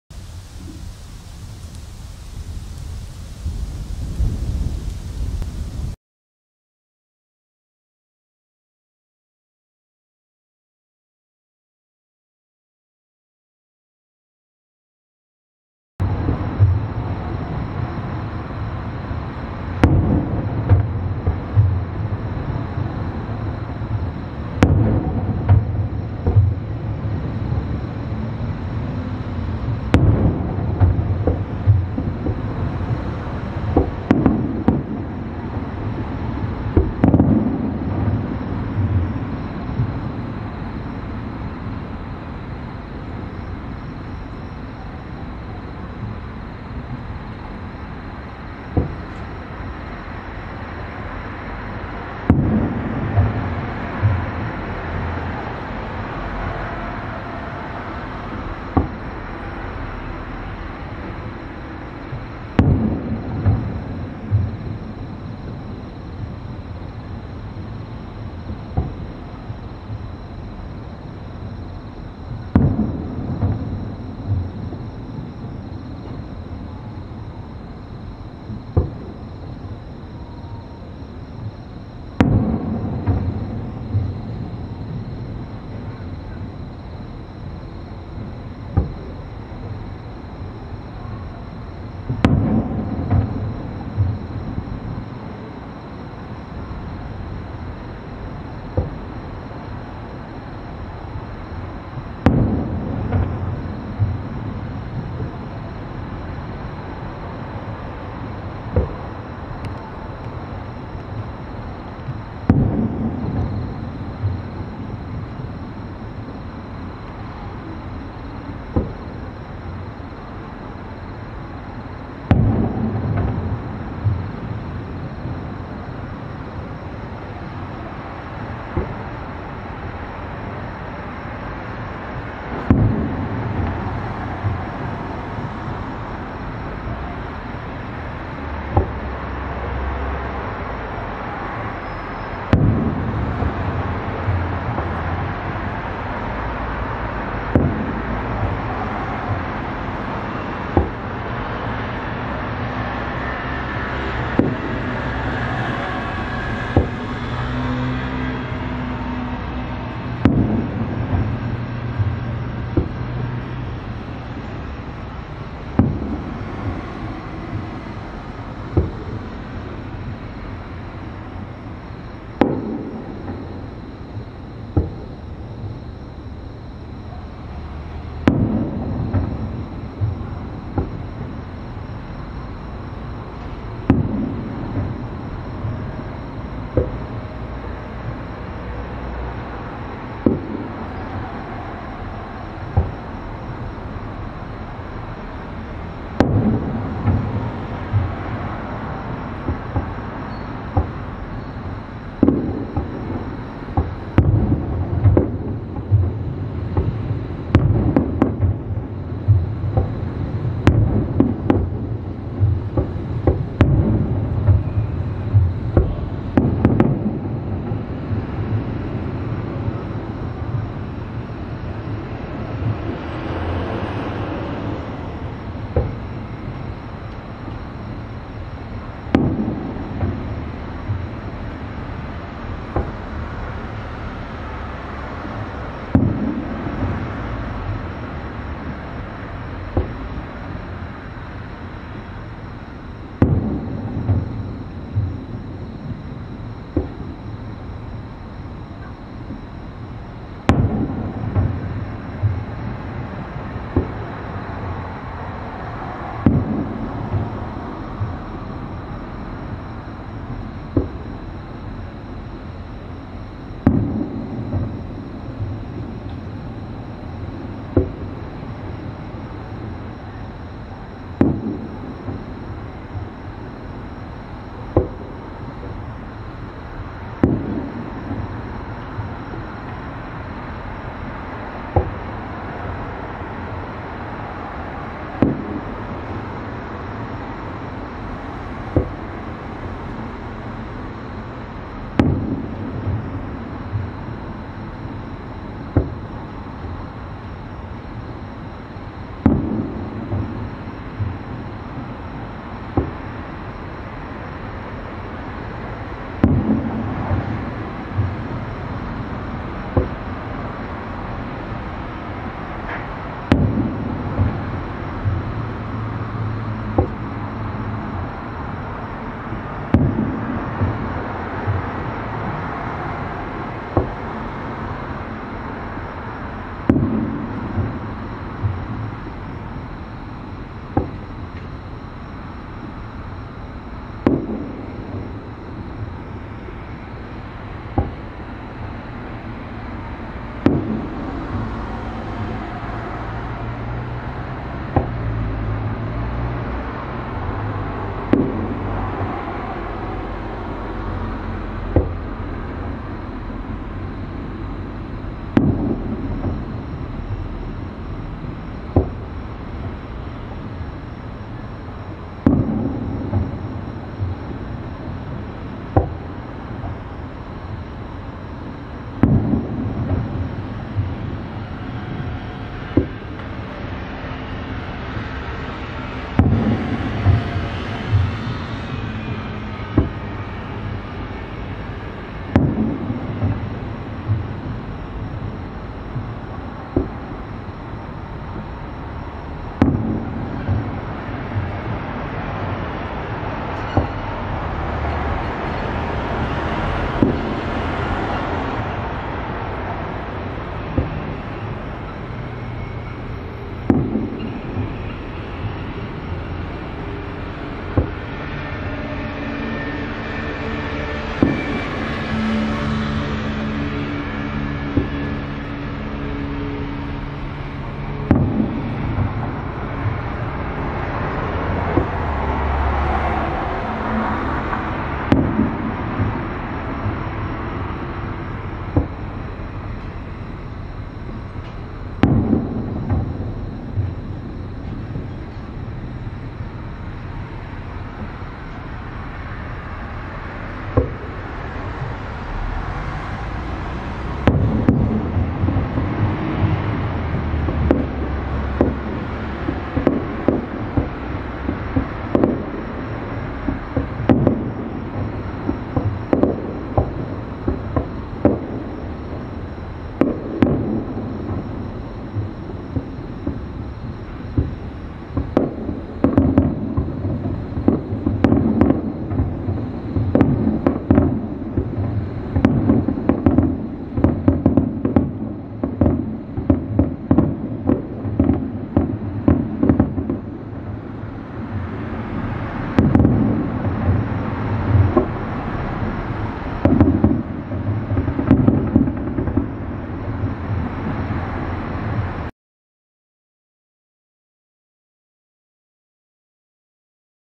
70代その日暮らしYoutuber@tokkun73 撮れたてサラダ Vol.5 花火 2021/08/25 uncut
音がするから何かなと思ったら、ベランダに出てみると西の空に花火が！
今年どこも行けなかった貴方にプレゼント！フィナーレが圧巻です。
つい1時間ほど前に撮影しました。ご覧ください！